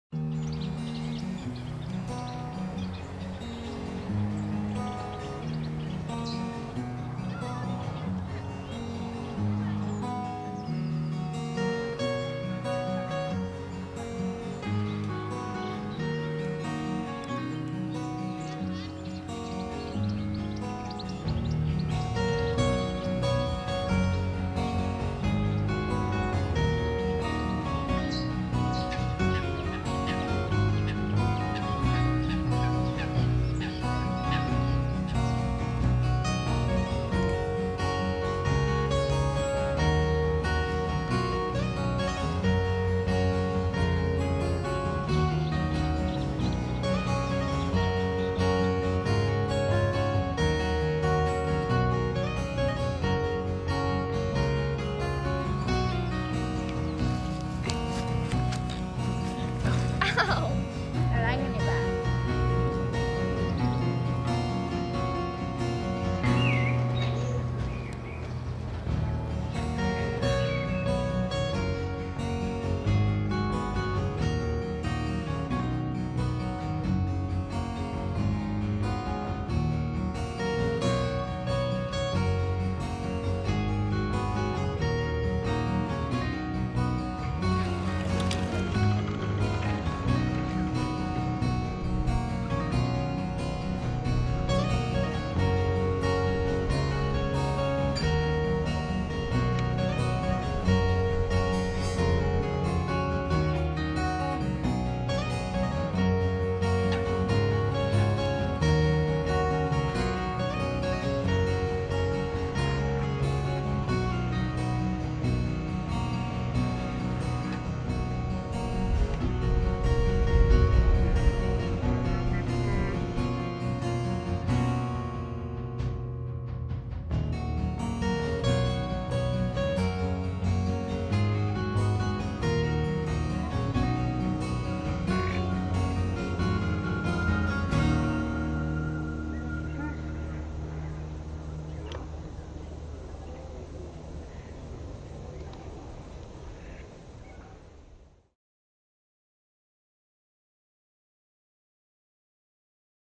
Olympic Park London - Sand Martins, Soldier Beetle, Damselflies Canada Goose Chicks and a HybridS
A few wildlife shots on a short cycle around the Olympic Park, in Stratford, London.
A Soldier Beetle, Banded demoiselles (female and male), Crane Fly, Coots having a fight (it wouldn't be Spring without a Coot fight), Canada Geese and their chicks, a Canada/Greylag hybrid and Sand martins collecting nesting material.